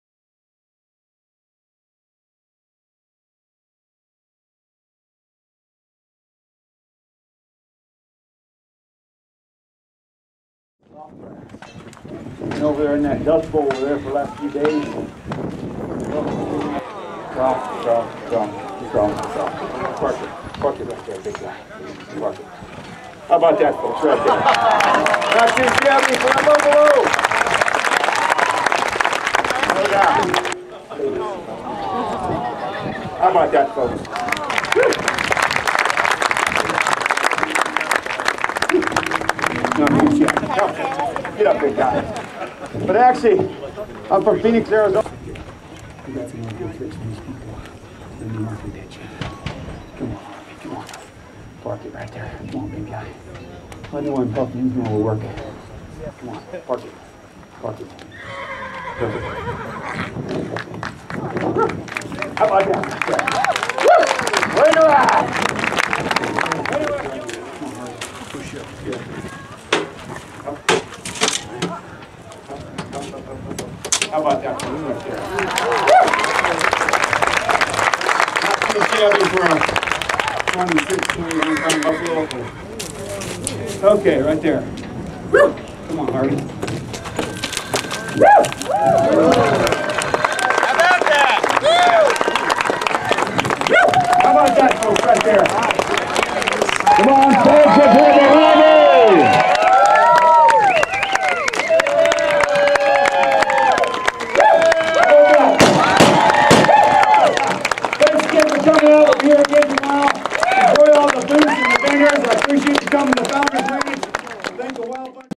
Buffalo Express
T C Thorstenson riding "Harvey Wallbanger " at SASS End of Trail, Founders Ranch, New Mexico 2008